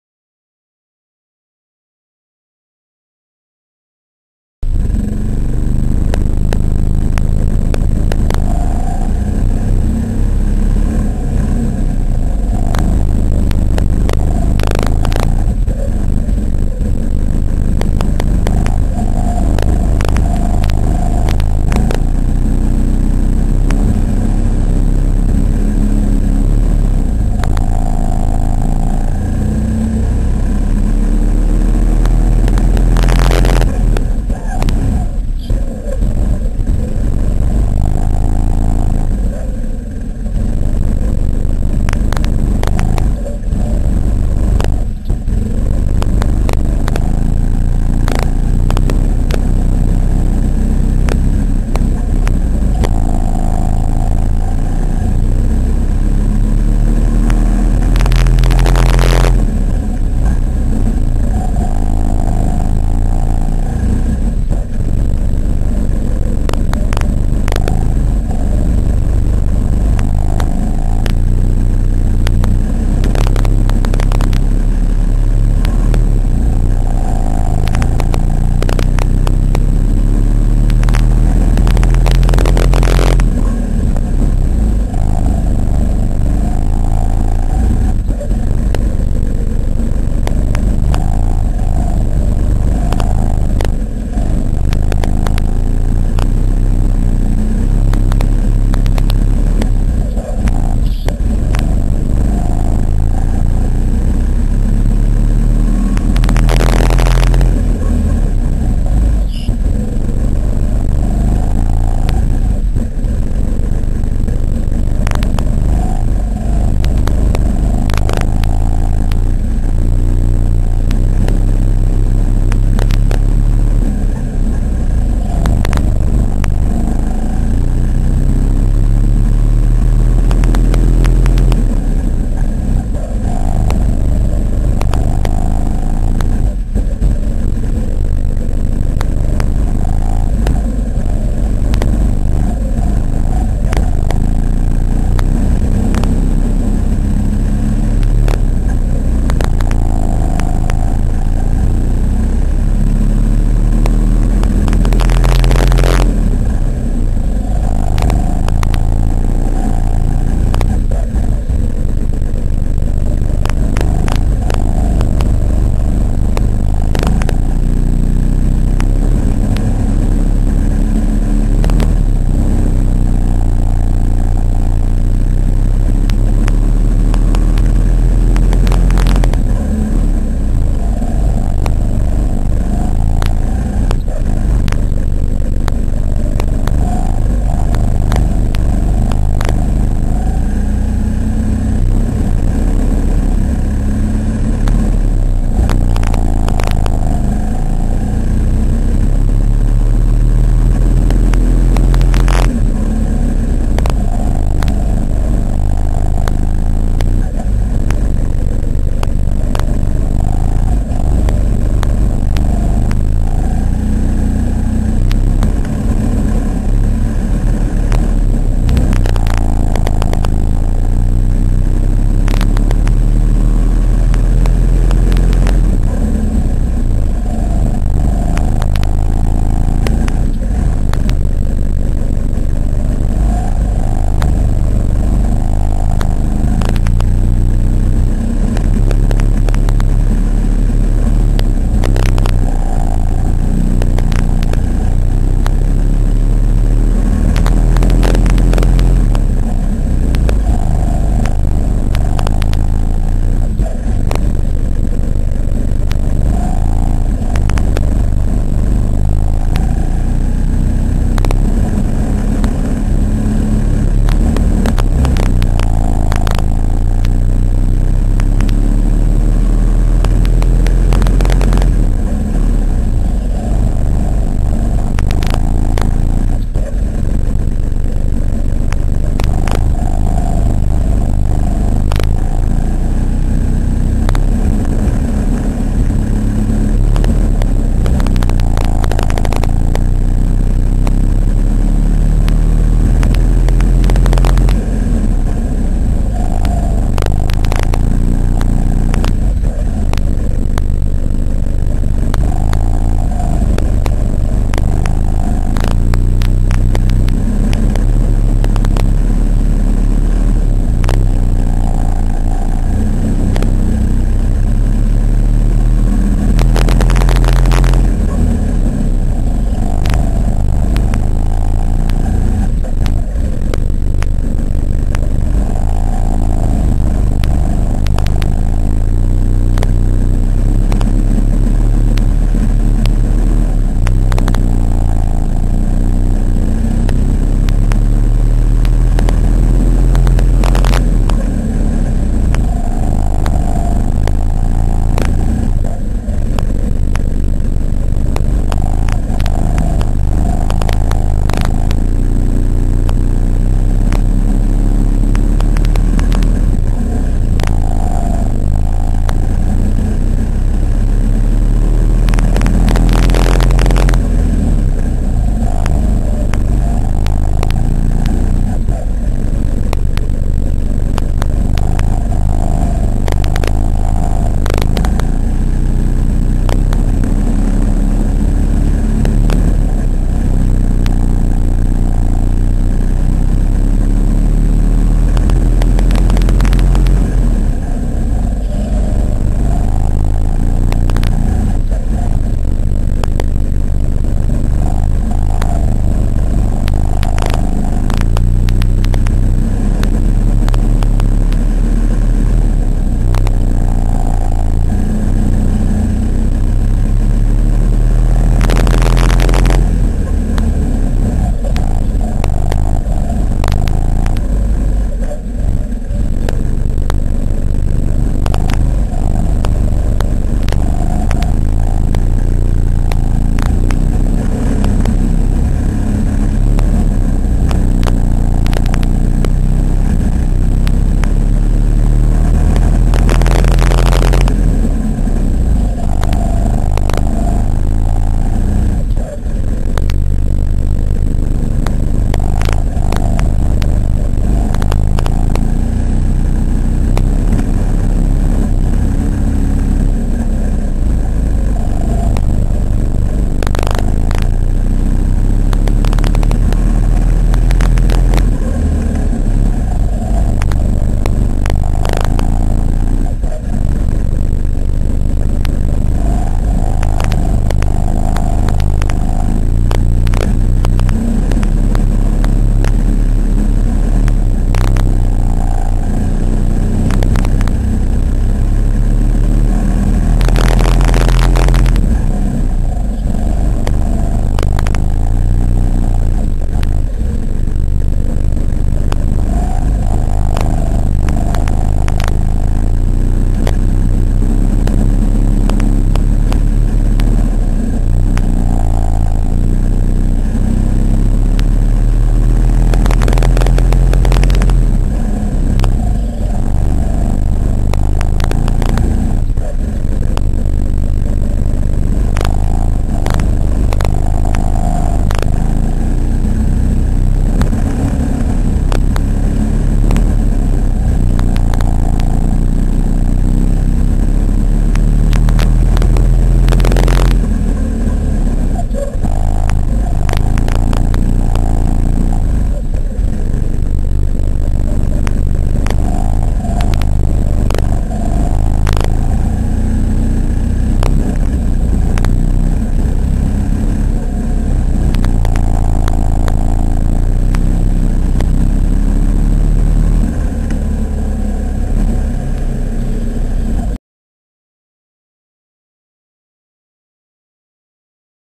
Indoor Karting @ Allsports (Feb. 28, 2009) - John
Meetup at Allsports GP Indoor Kart Track (2/28/2009).  for more info.